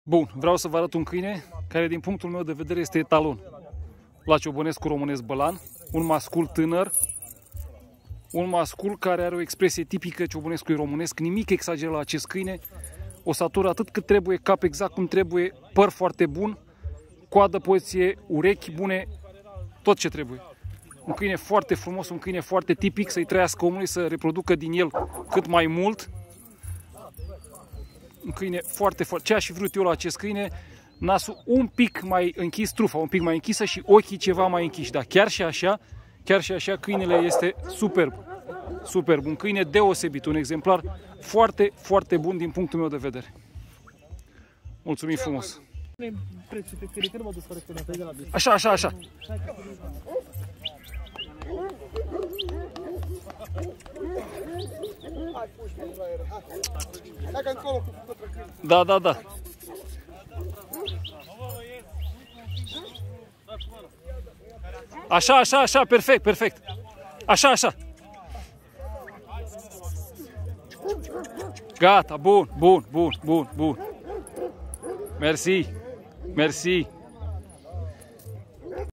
Ciobănesc românesc bălan - masculul Gruia din Mureș, prezentat de Ștefan Nemeș din Maramureș
Un mascul deosebit din punctul meu de vedere.
Îl aștept cu drag în viitoarele expoziții chinologice organizate de către UChR.